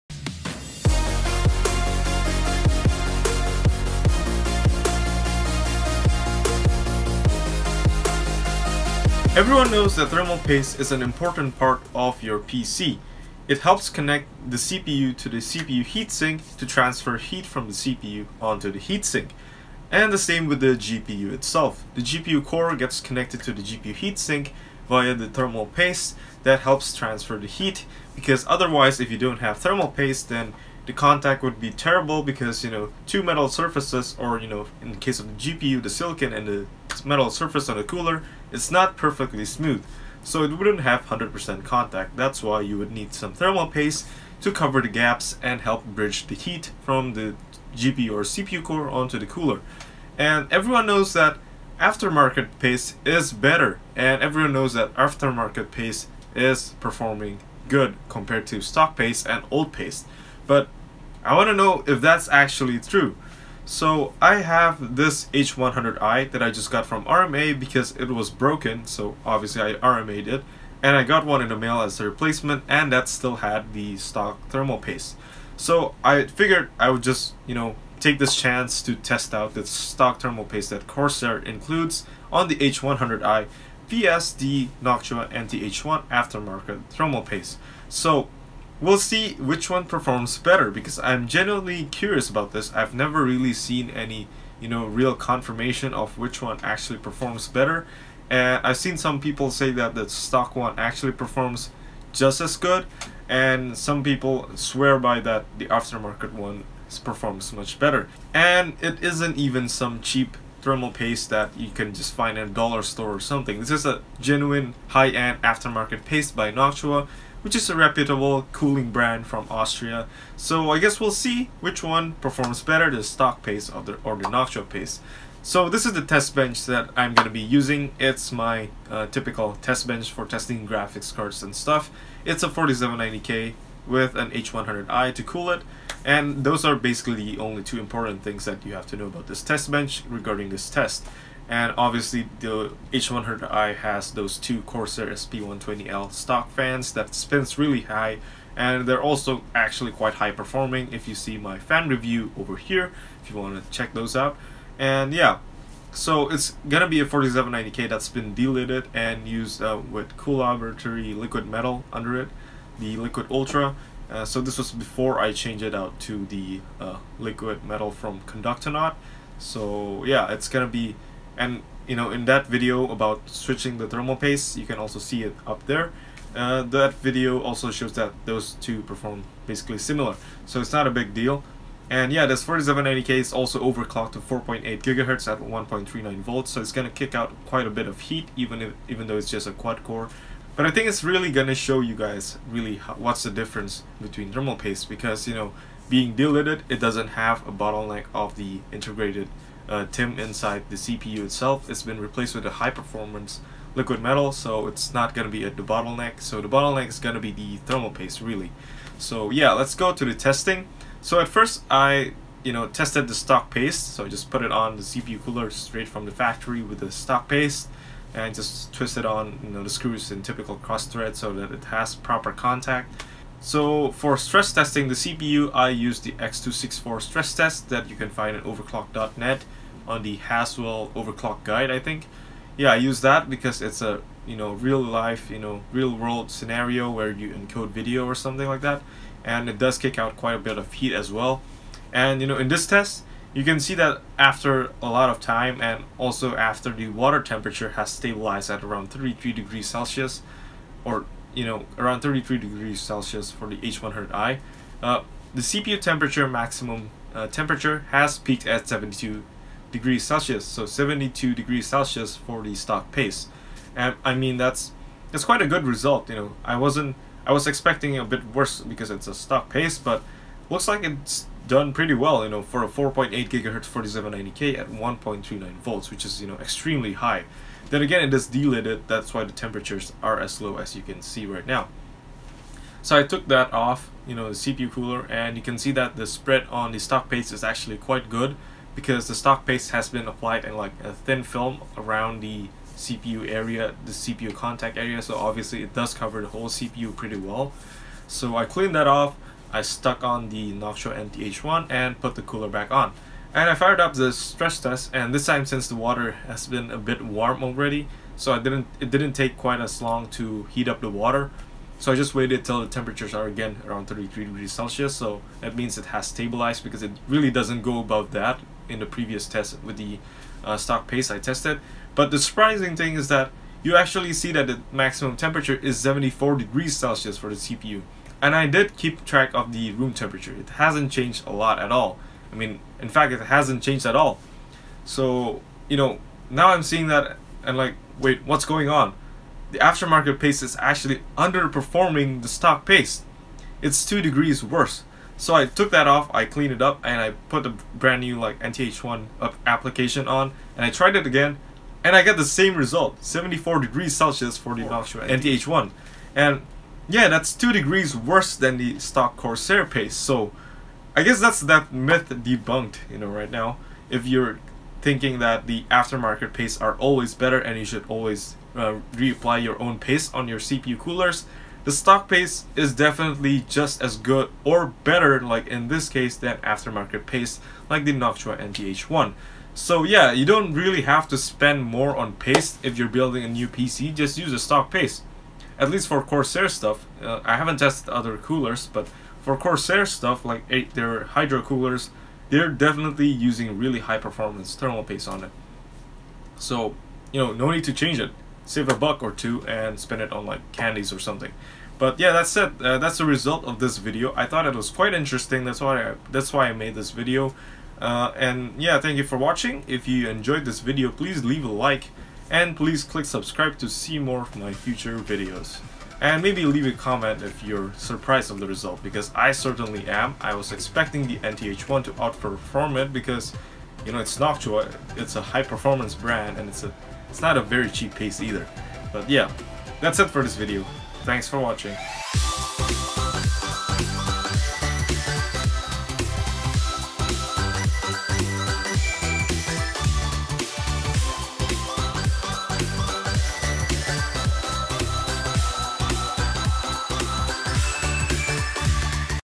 Don't change your thermal paste! | Corsair Pre-applied VS Noctua NT-H1
Since I got my H100i V2 from RMA, I thought this might be a great time to test out to see if the pre-applied paste that Corsair supplies with their Hydro coolers are any good, so I compared it to a high end Noctua NT-H1. Is it as good as an aftermarket paste? Do you actually have to replace your thermal paste? Well if you read the title...then you know that the answer will surprise you.